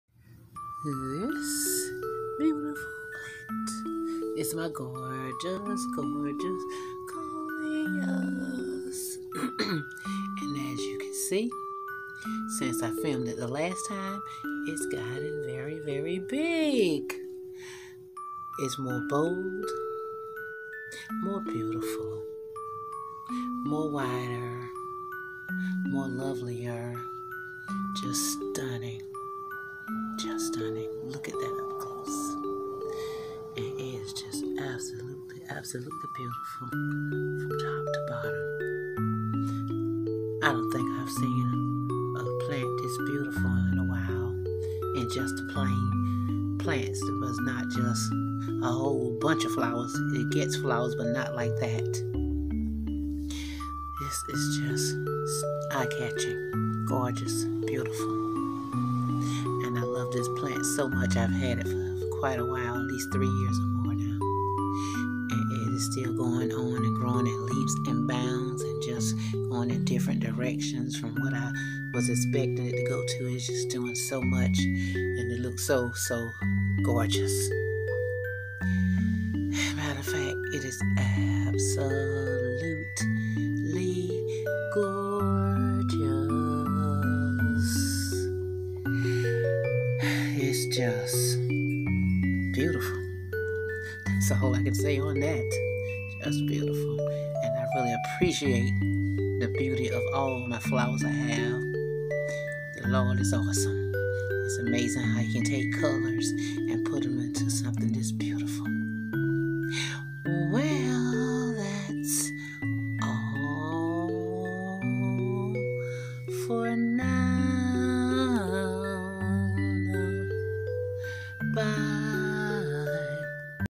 My Coleous Is Absolutely Breathtaking!!
Bold And Beautiful Multi Color Flower It's Stunning In Color It Absolutely Breathtaking! blackgardener beautifulcolors # breathtakingyetstunning